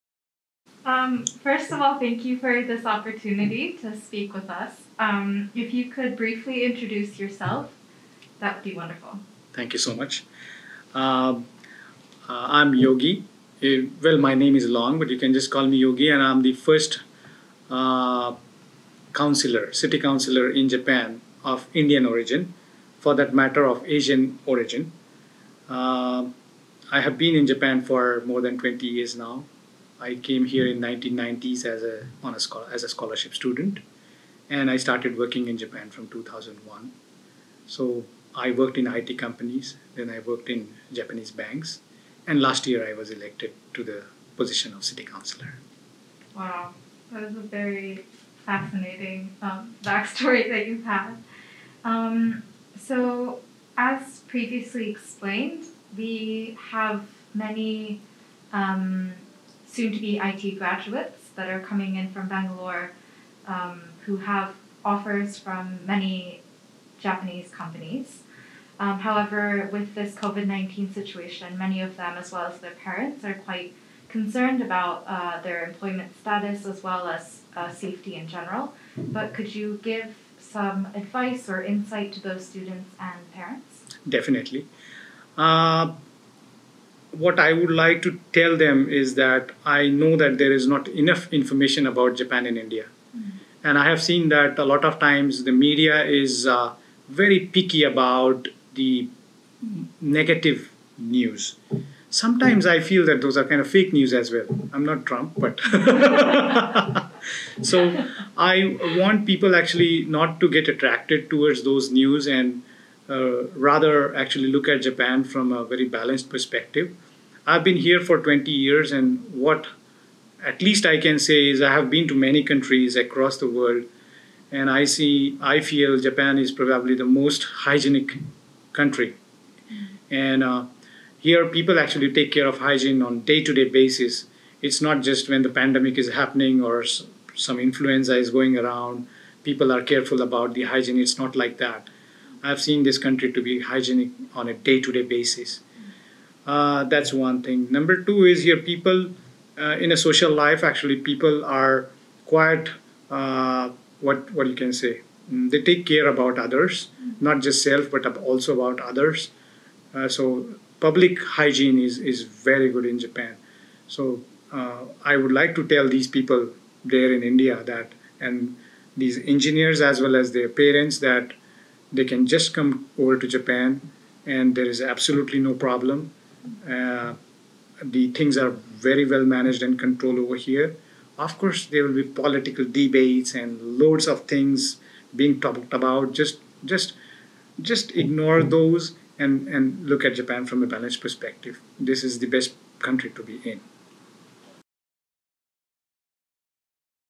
Yogi Zenken interview, 20-May-2020
Edogawa City Councillor Yogi (Yogendra Puranik) Zenken interview, 20-May-2020
2020年5月20日、江戸川区議会議員よぎ（プラニク・ヨゲンドラ）、全研本社でのインタビュー。